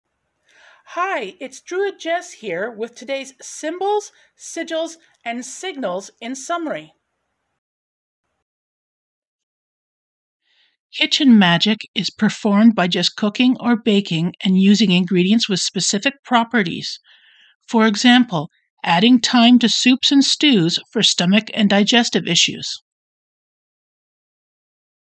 Druid Jess Symbols, Sigils and Signals in Summery – Kitchen Magick
In a quick summery, Druid Jess tells you about the correspondences of symbols, sigils and signals in relation to the Pagan belief system. This series is for all audiences. Photo of Druid Jess was provided by Alexis Allinson Logo for Spiritual Connections was a collaboration of artistic vision of Mike Allinson, Druid Jess, Peter Lami, Aleena Craig and Alexis Allinson. Logo was created by Alexis Allinson. Photo provided by Pixource located on Pixabay.com